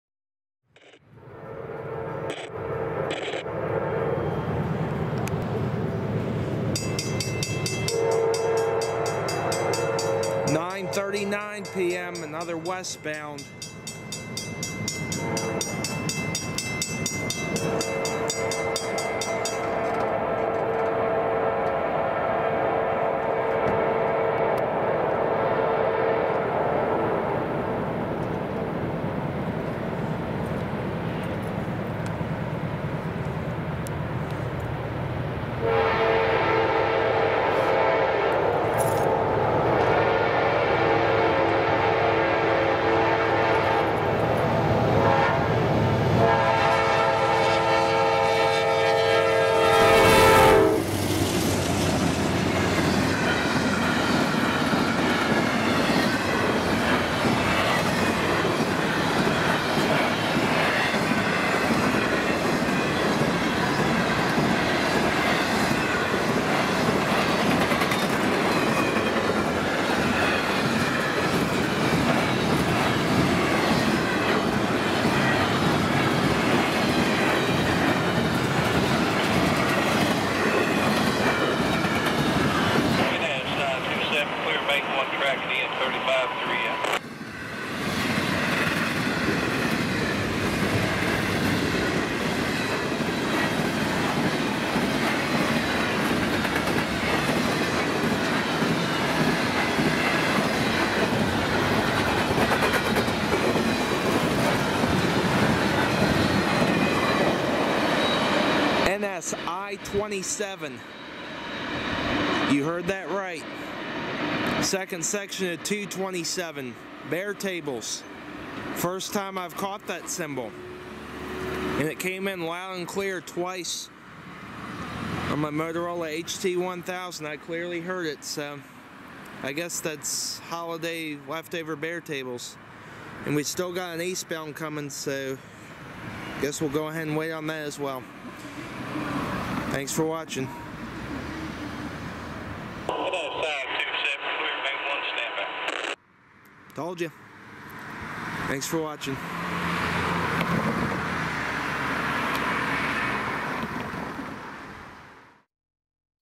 NS I27
9:39 PM

NS I27 heads west as an all baretable move with a trio of GEs. These are likely leftover holiday baretables. Shot in 4K with a Panasonic HC-VX870.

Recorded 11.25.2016